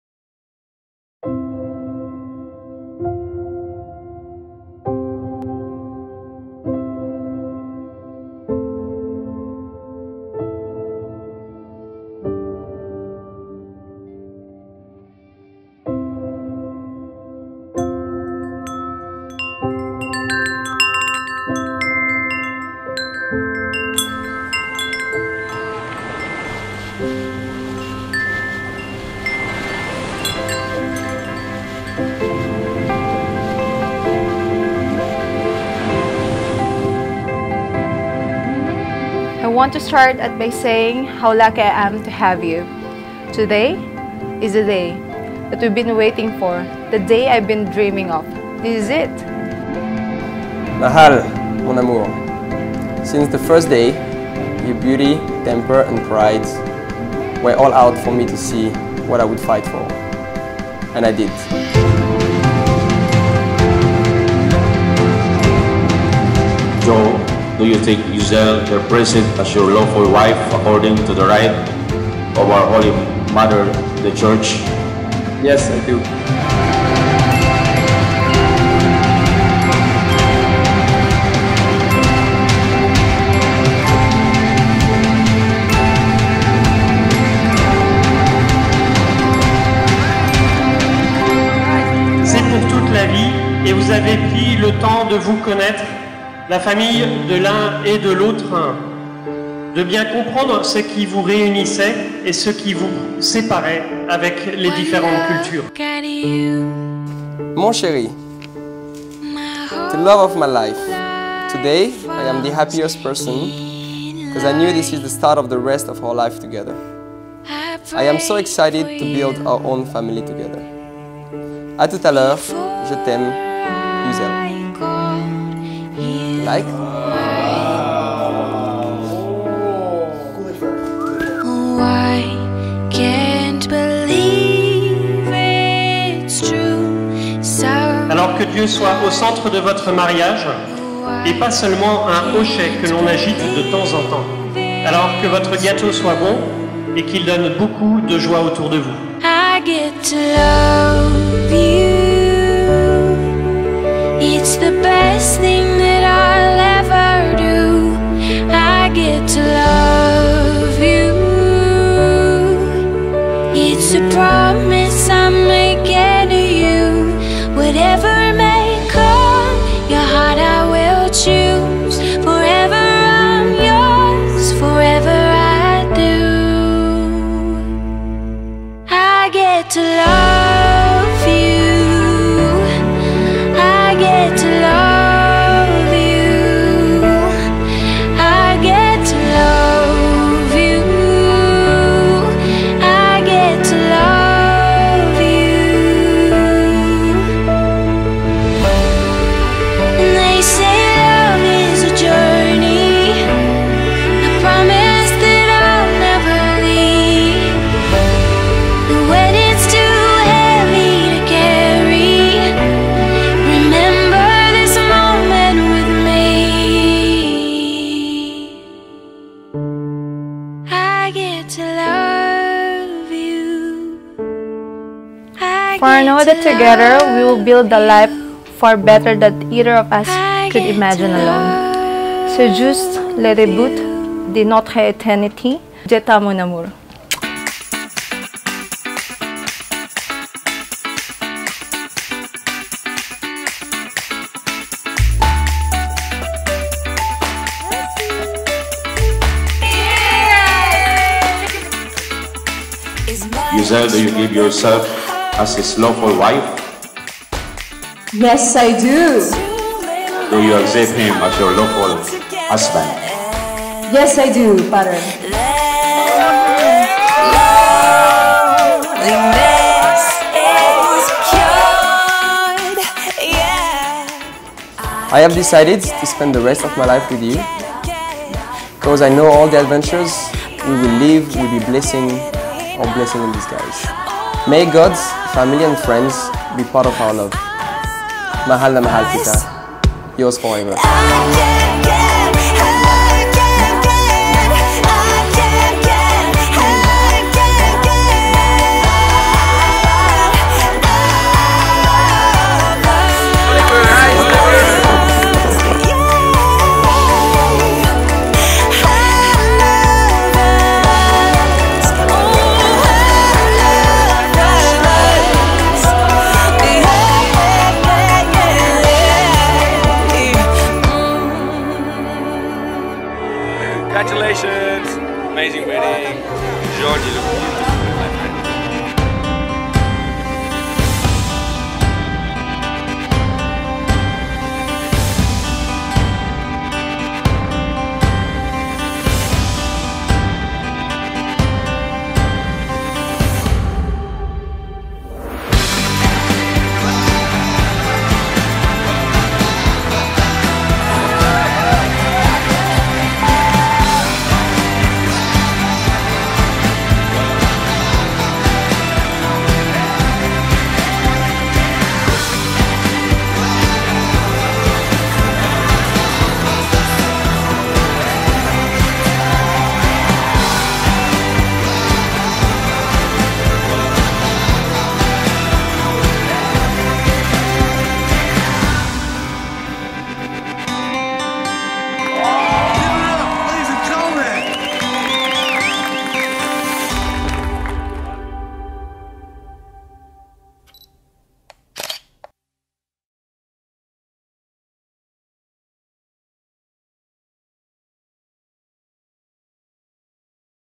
JEAN + UZZIEL A Beautiful Same Day Edit Wedding Film in Calatagan Batangas
SDE VIDEO EDITOR: JOCES MACERES